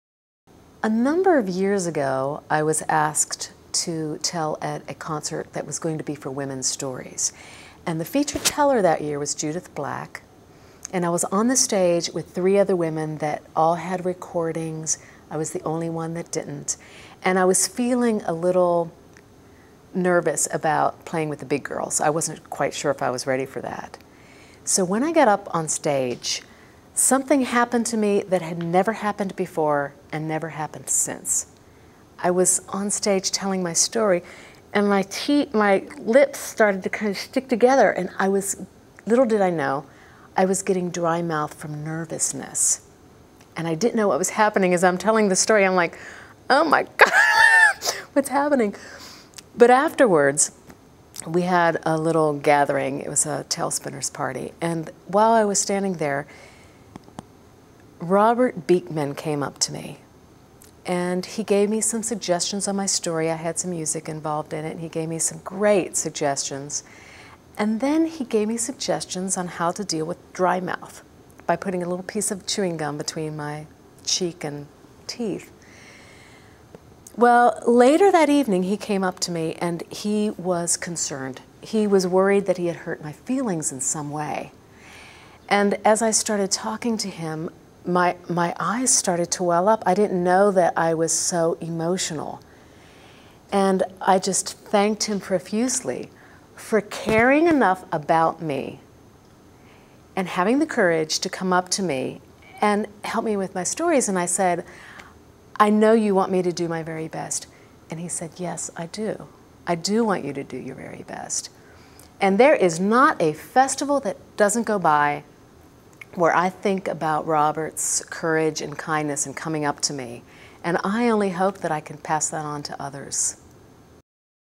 kim - thank you robert
Texas Storytelling Festival celebrates 25 years of stories, memories, and friendships. Interviews by Jim Cullers and Richard Croxdale along with Kim Lehman at the TSA convention, summer 2009.